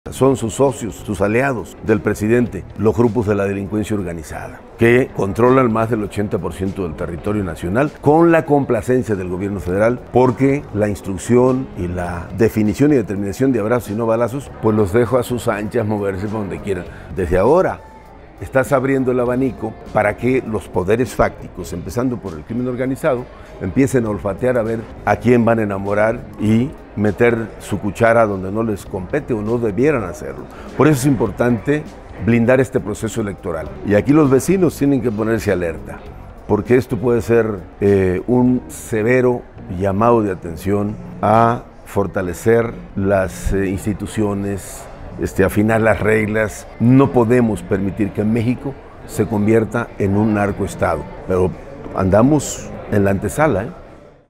"Los grupos de la delincuencia organizada son aliados de AMLO", SILVANO AUREOLES
El aspirante del PRD a la coordinación del Frente Amplio, Silvano Aureoles, dice en entrevista con Expansión que los grupos de delincuencia armada son socios y aliados del presidente de México Andrés Manuel López Obrador.